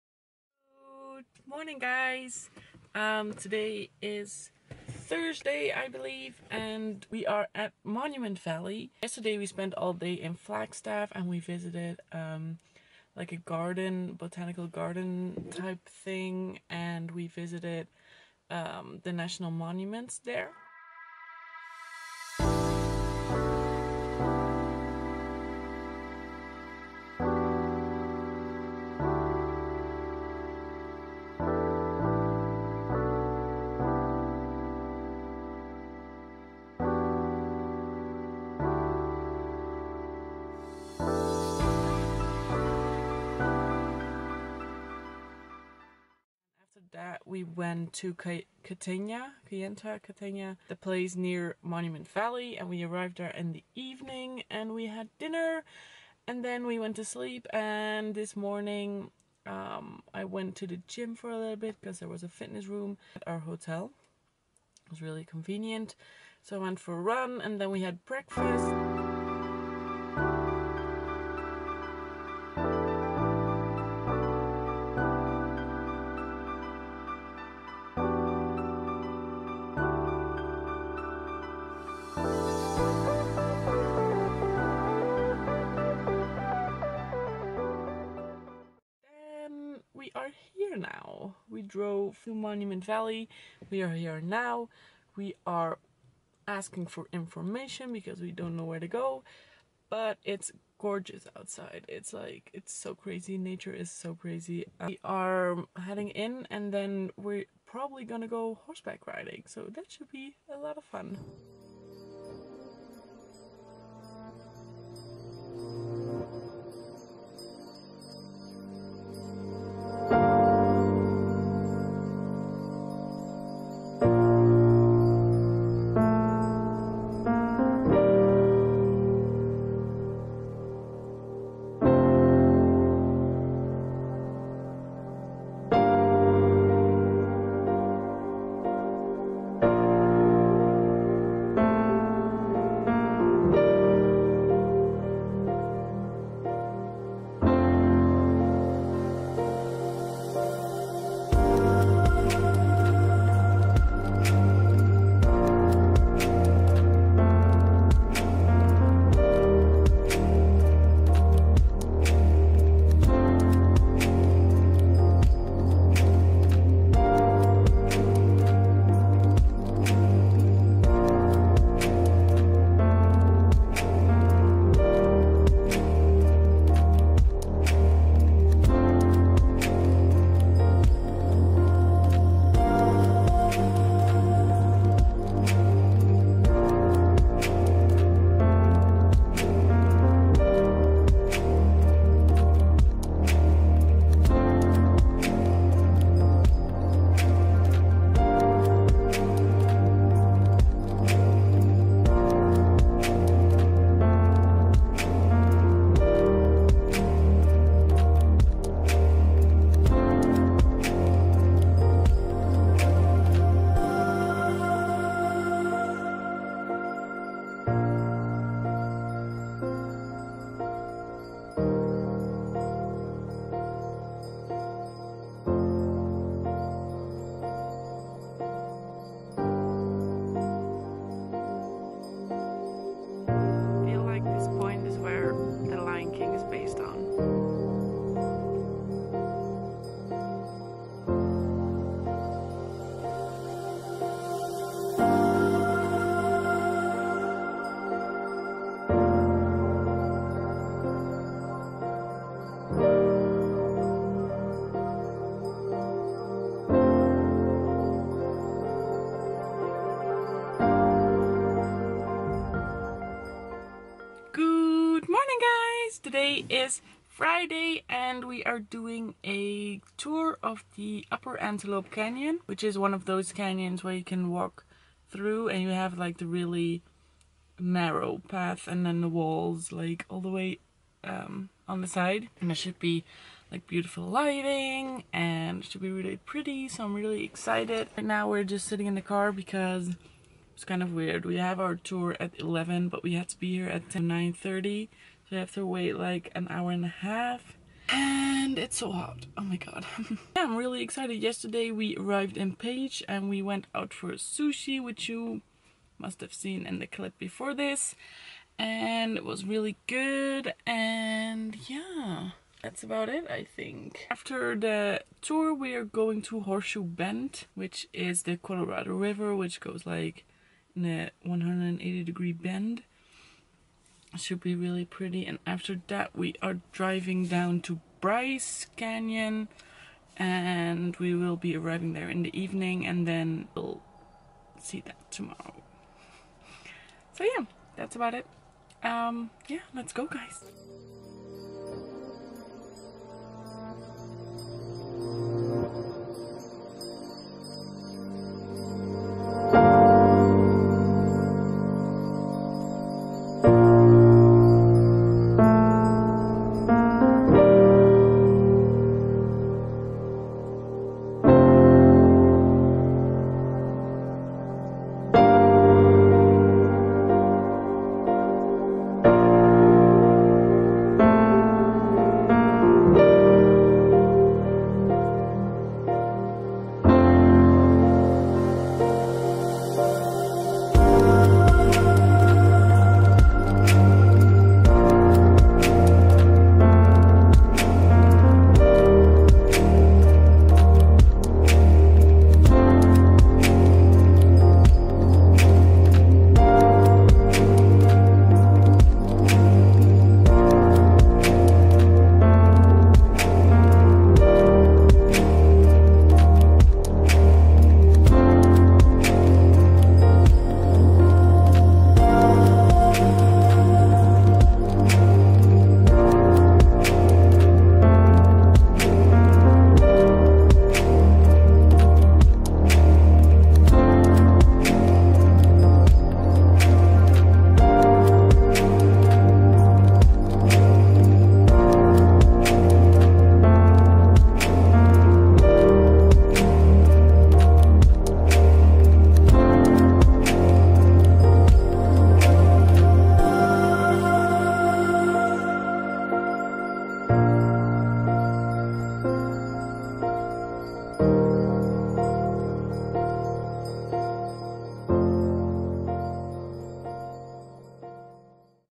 VLOG 44 - HORSEBACK RIDING + ANTELOPE CANYON
VLOG #44 - We went horseback riding through Monument Valley, did a Navajo tour thorugh Upper Antelope Canyon and went to see Horseshoe bend